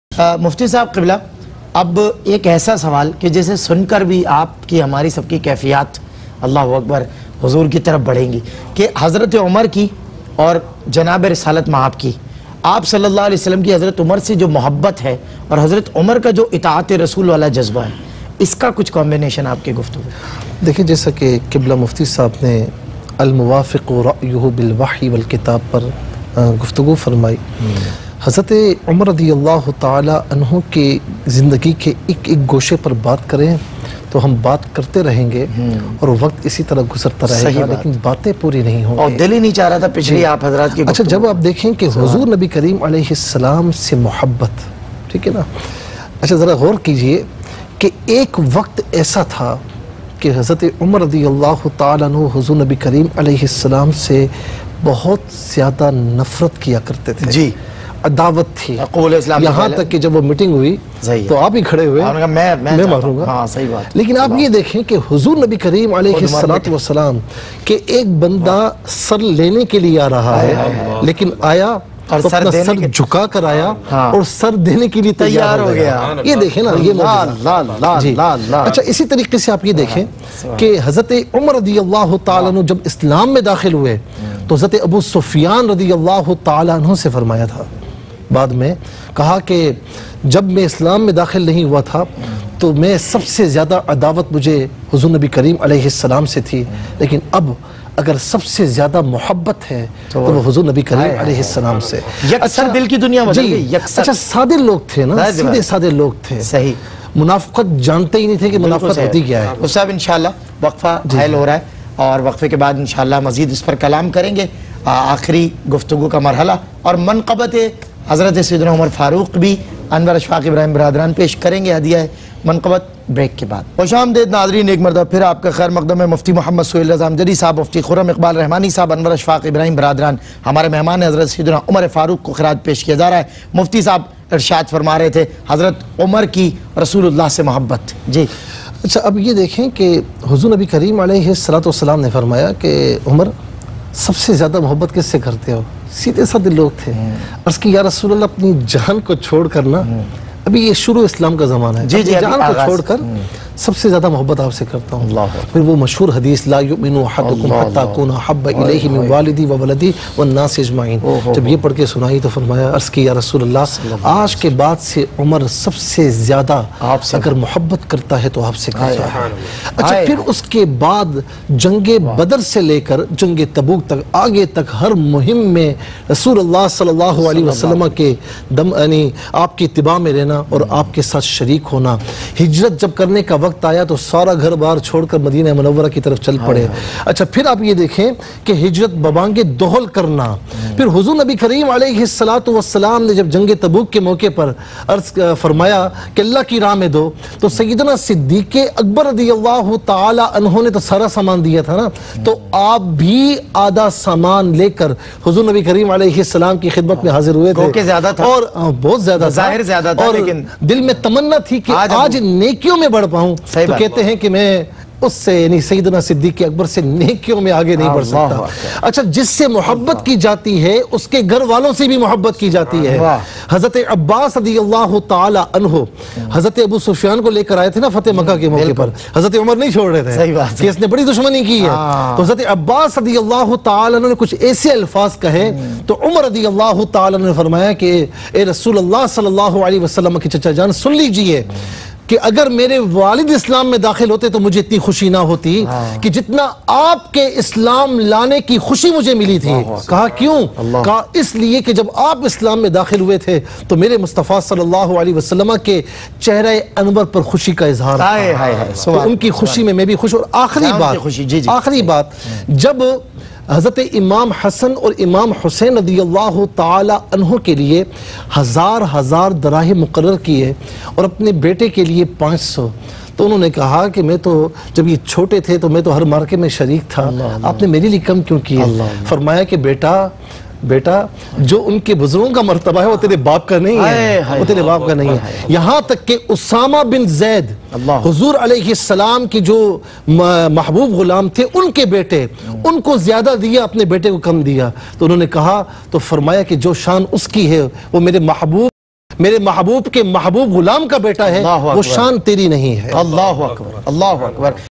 Hazrat Umar Farooq RA Ki Huzoor Nabi KAreem SAW Se Nisbat Aur Muhabbat - Mufti Muhammad Sohail Raza
Al Farooq R.A - Hazrat Umar R.A Special
Host: Syed Salman Gull - Sana Khuwan: Anwar Ibrahim & Ashfaq Ibrahim
Guest: Muhammad Khurram Iqbal Rehmani - Mufti Muhammad Sohail Raza Amjadi